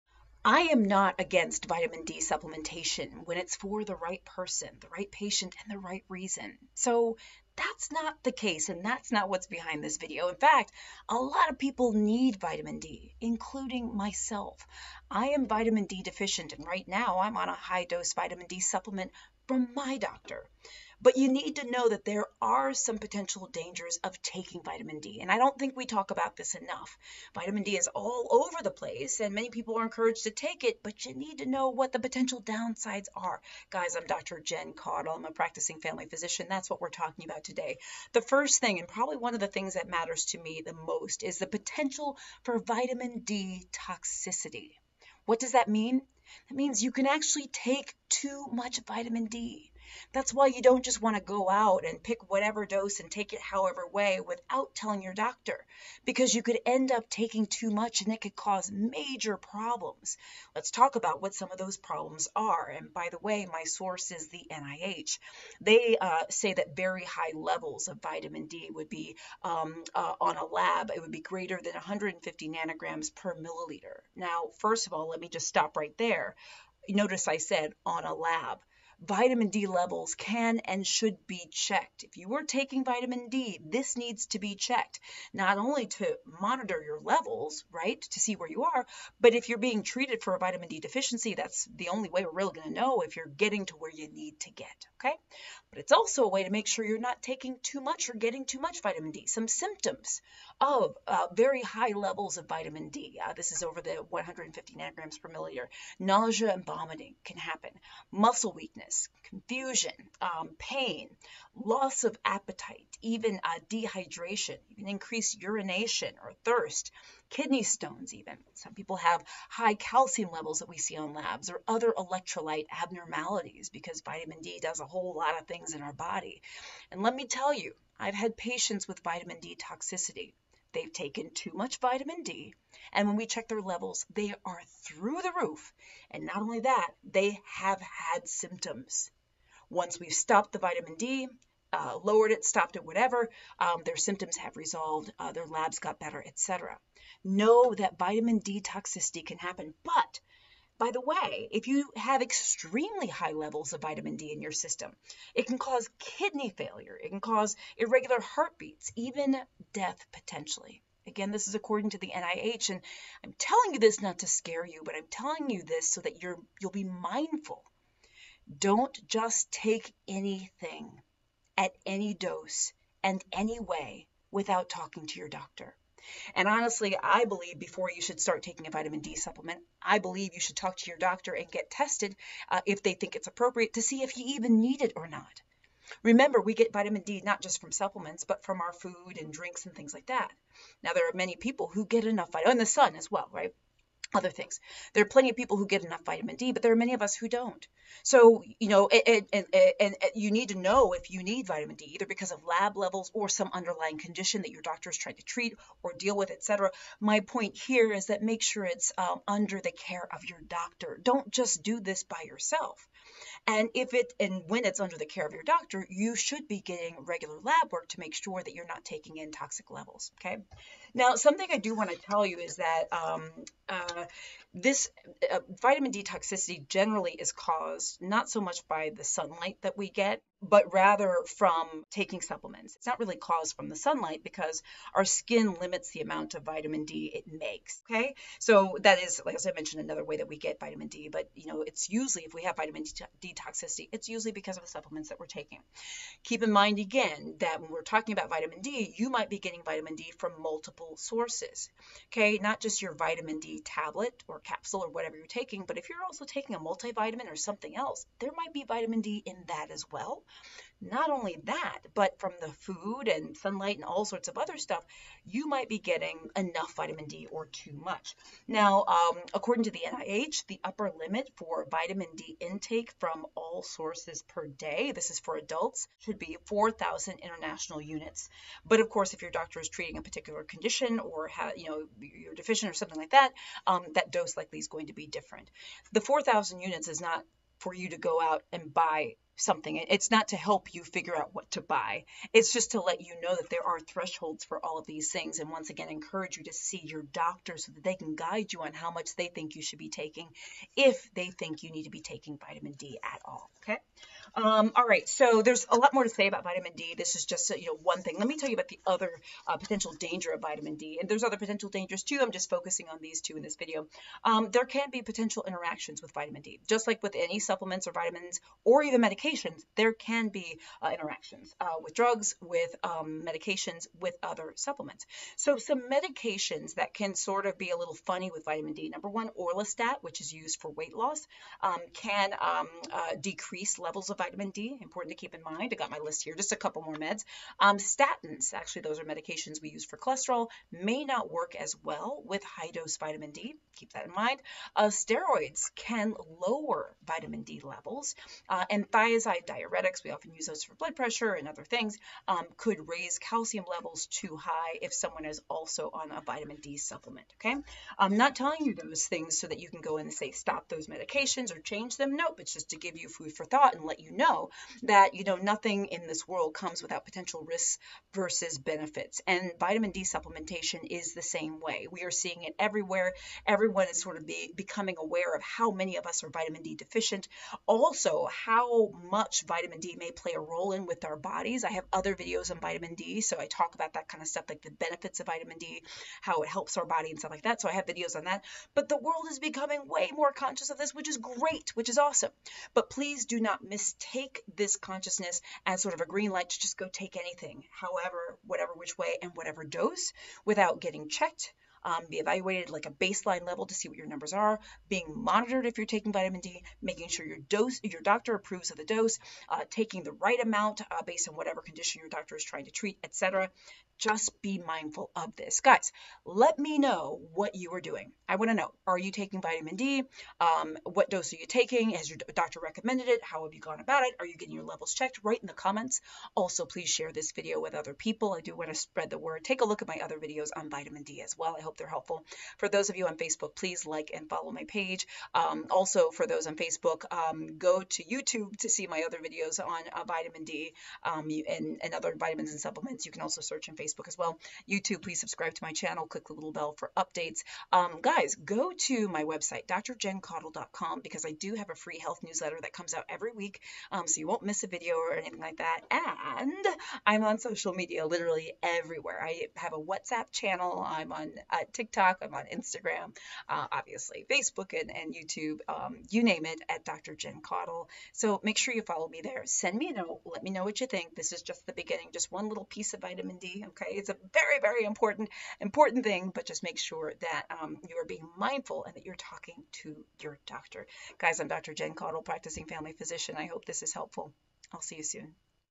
The DANGERS of VITAMIN D Supplements! A Doctor Explains What You Should Know!
Vitamin D is important for many, but it’s not without its problems. Take a look at this video to learn more!